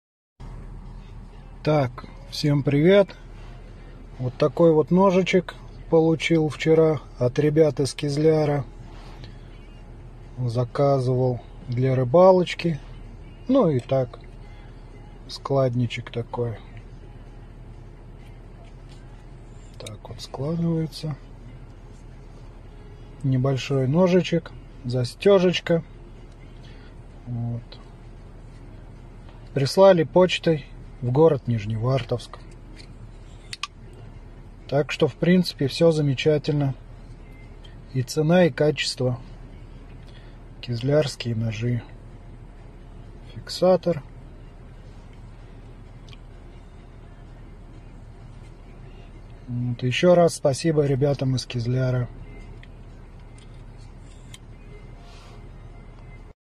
Кизлярские ножи / Складной нож Ирбис ООО ПП КИЗЛЯР /ОТЗЫВ от нашего уважаемого клиента Нижневартовск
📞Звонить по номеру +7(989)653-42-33